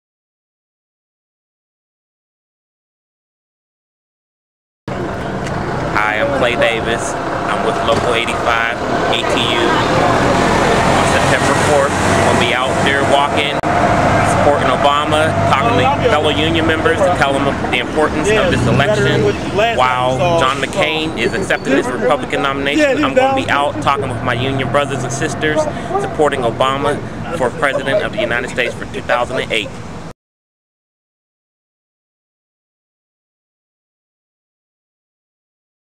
PA: ATU Local 85 on September 4th Labor Walks
ATU Local 85 member Clay Davis talks about why it's important that members participate in the September 4th labor walks.